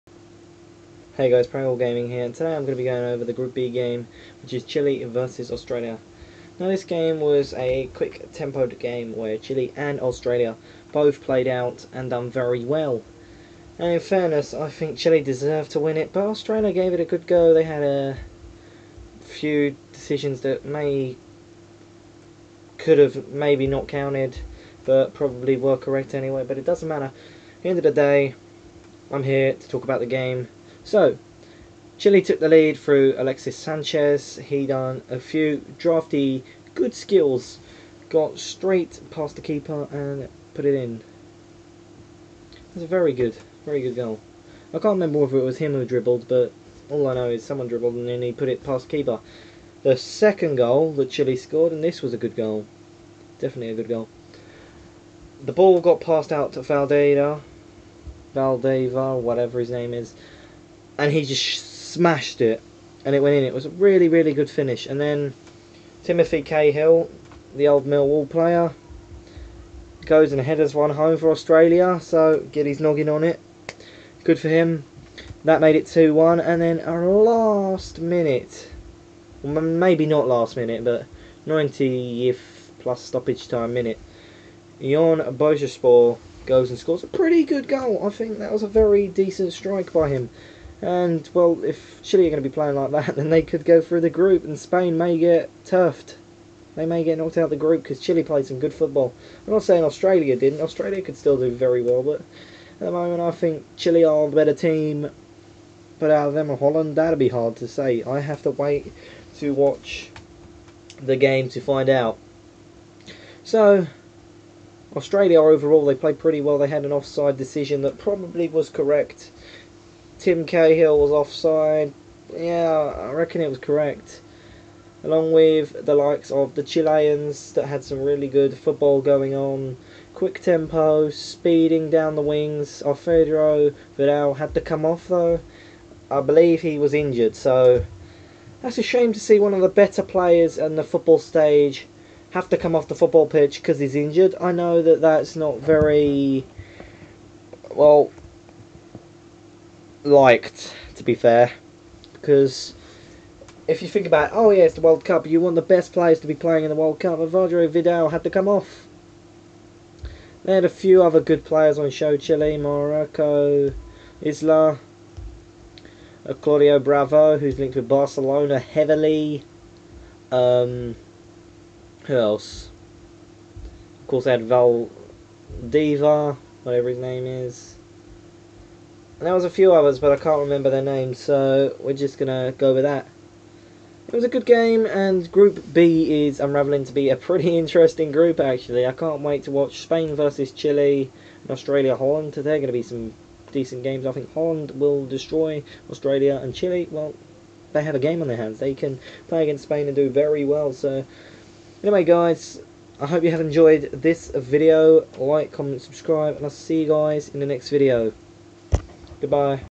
Brazil World Cup 2014-Chile V Australia after game analysis
The world Cup is in here it is Today, so I will be going over Chile V Australia

My Gamertag-
Prangell